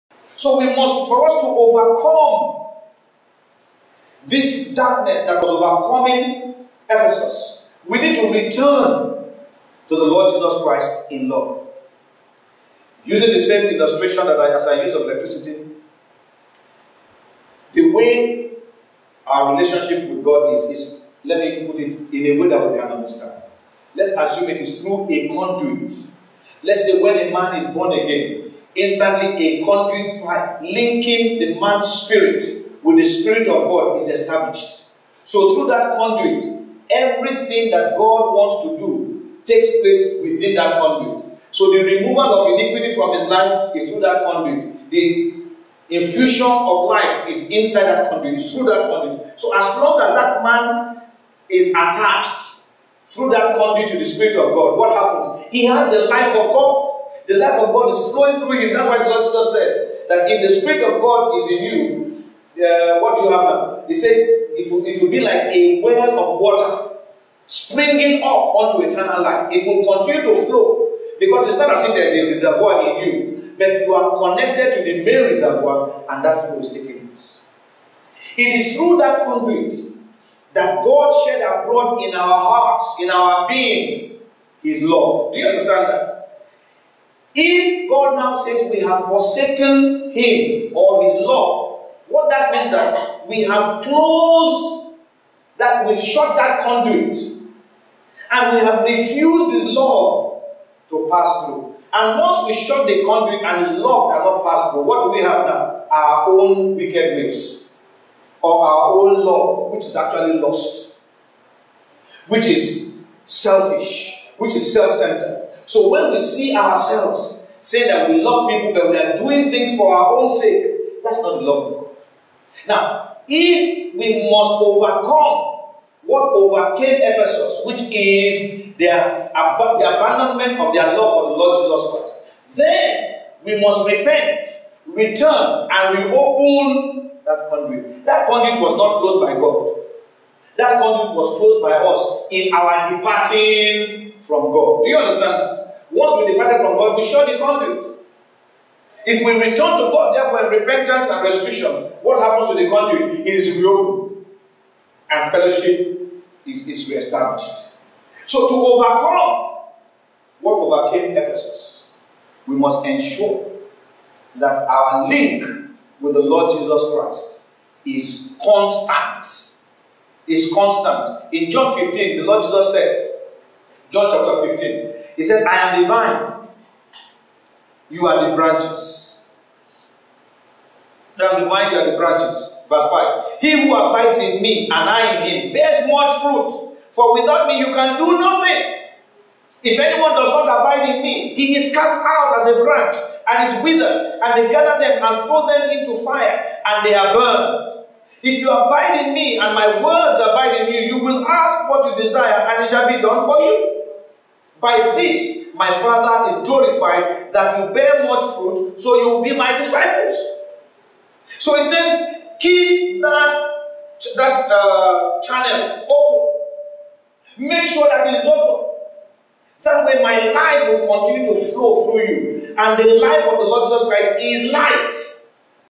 Overcoming What Overcame Ephesus Expounded
We give an illustration to help explain what it will take for us to overcome what overcame Ephesus.